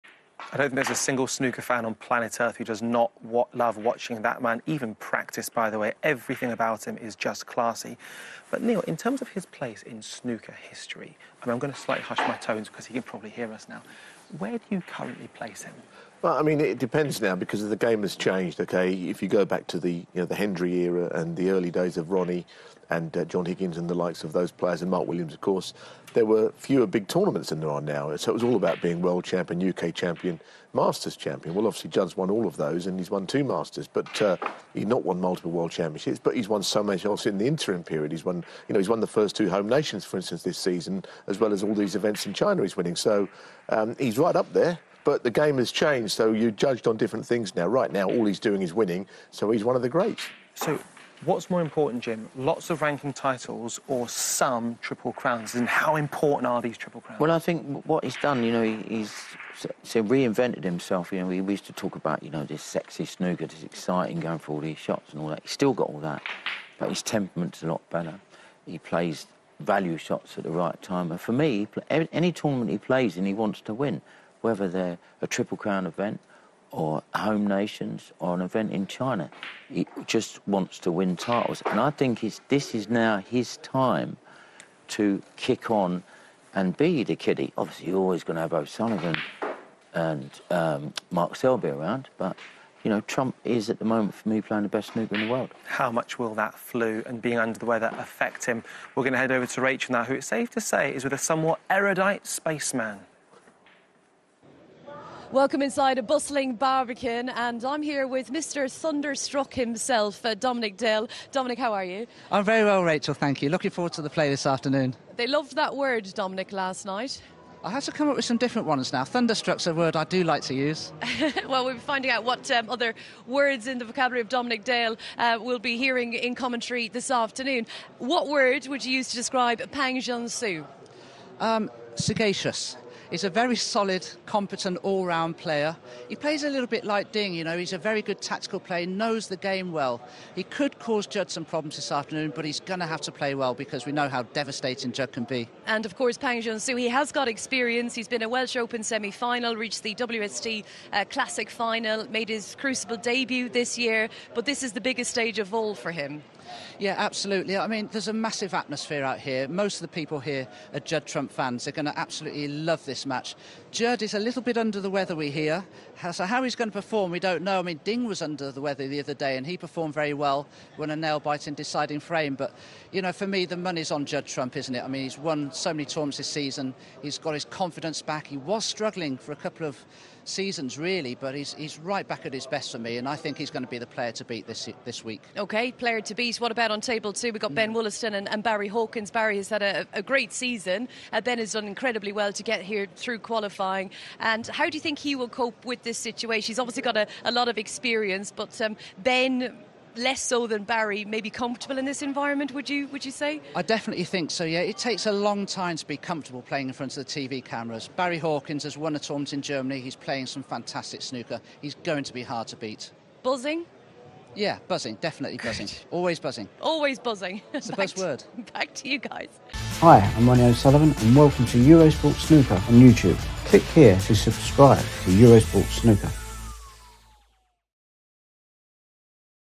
"ONE OF THE GREATS" 🐐 | Judd Trump's place in Snooker history discussed | UK Championship Snooker
Judd Trump's place in snooker history discussed ahead of his opening UK Championship clash against Pang Junxu.